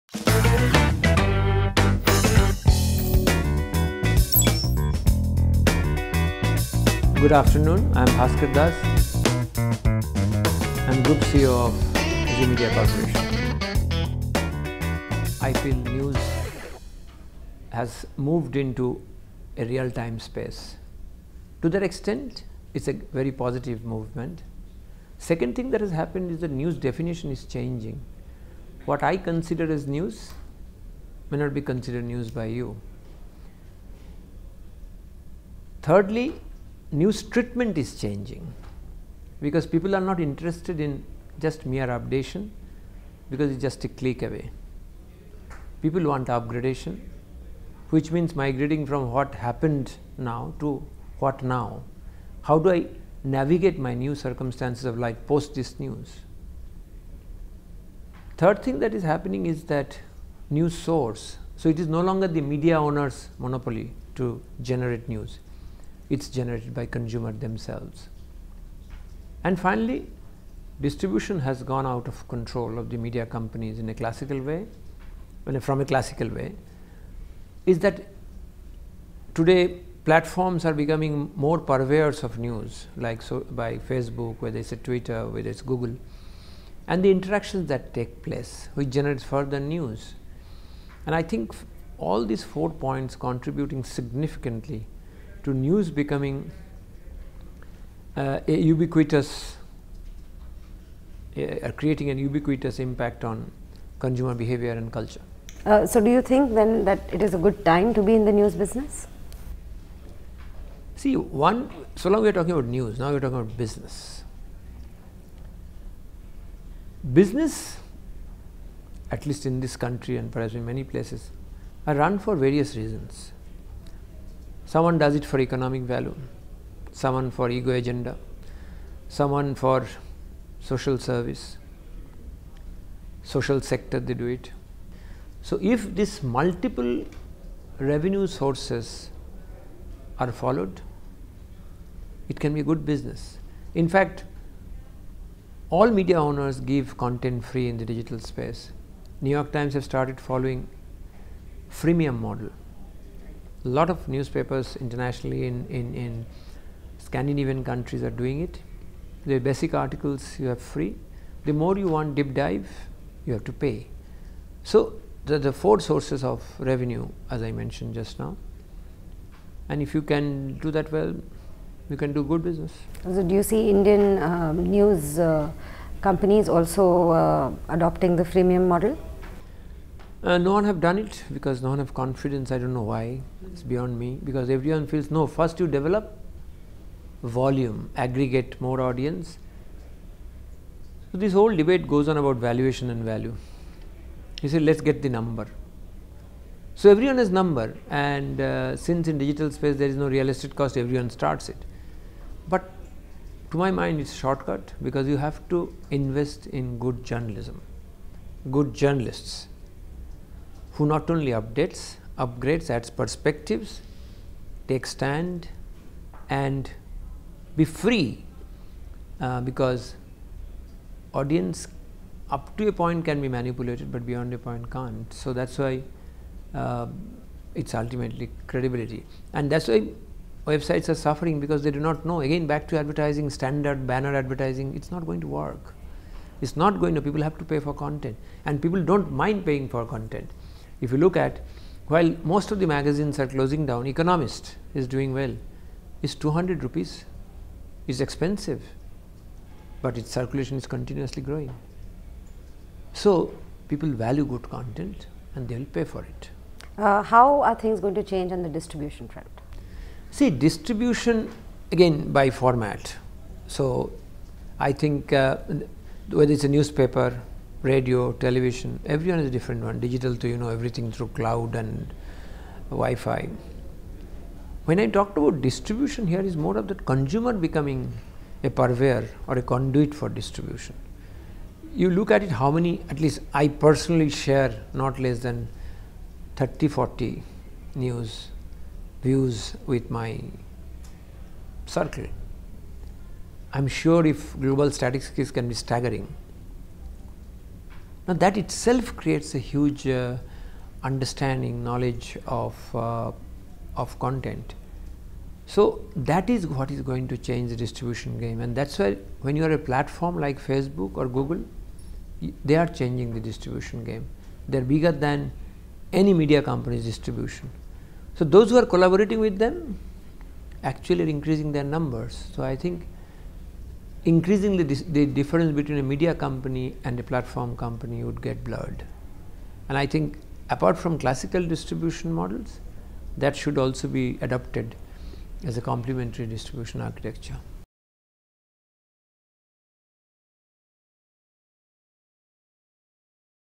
Up Close & Personal With Dr. Bhaskar Das || FICCI Frames 2016
Group CEO of Zee Media Corporation, Dr. Bhaskar Das talk about the change in consumption of news in an interview with Payal Bhattar, PING Network At FICCI Frames 2016.